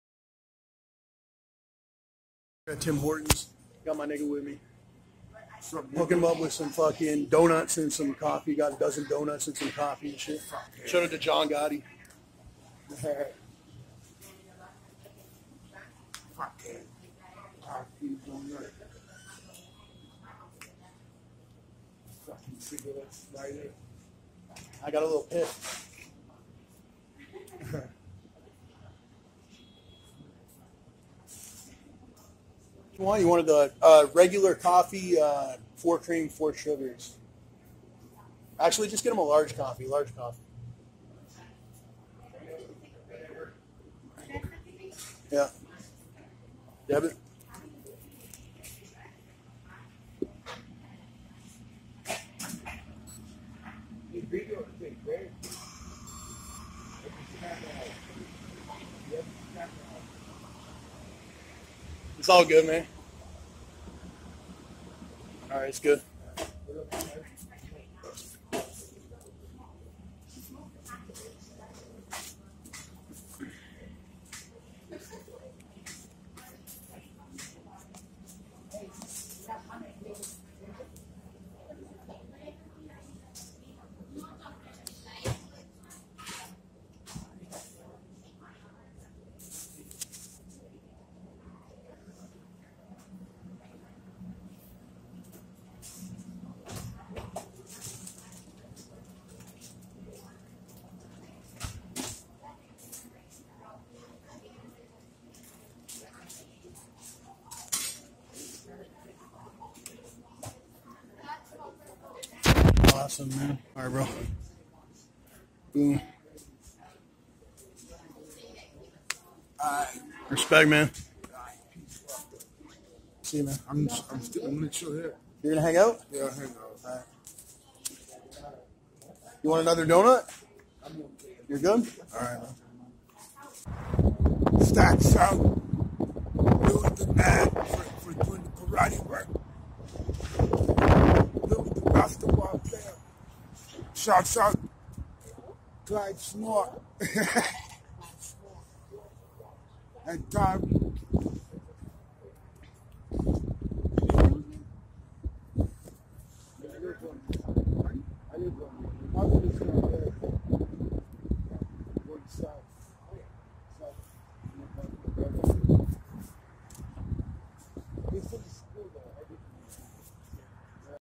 StacksAMilli Giving Away Donuts 🍩 & Coffee at Tim Hortons
Giving away donuts 🍩 and coffee to a few people in the neighborhood.. This one I got on camera. donuts coffee treats timhortons Shout out to @gongotti